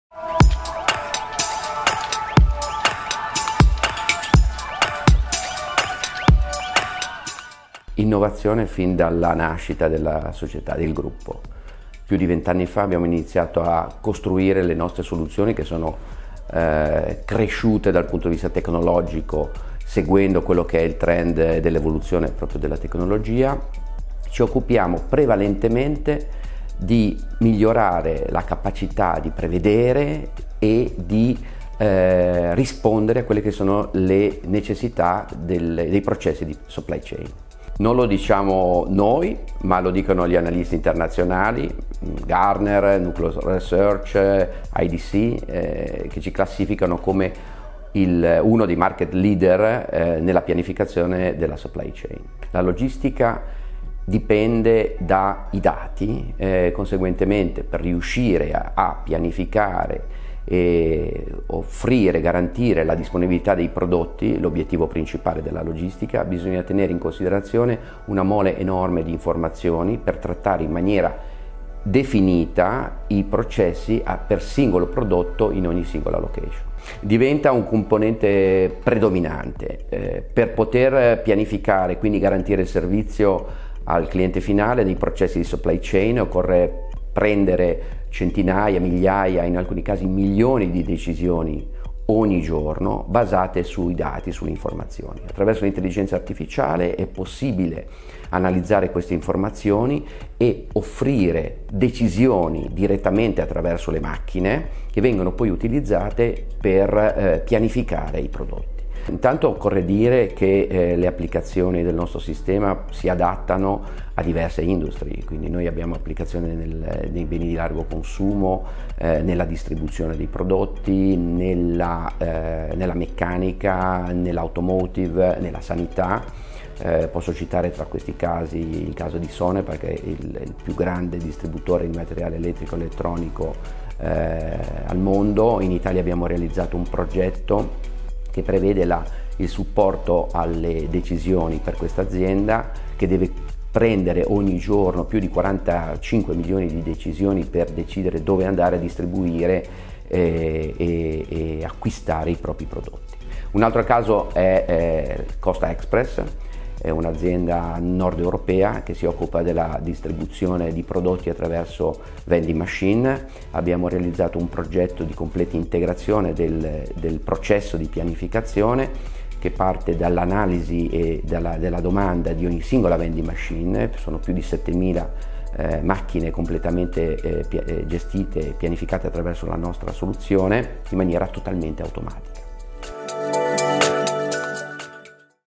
ToolsGroup, il software a servizio dell'efficienza della logistica
Francesco Stolfo, Vice President Business Development di ToolsGroup, descrive le caratteristiche e i punti di forza dei software di ToolsGroup, dedicati all'ottimizzazione della catena logistica. Mettendo in evidenza alcuni casi di successo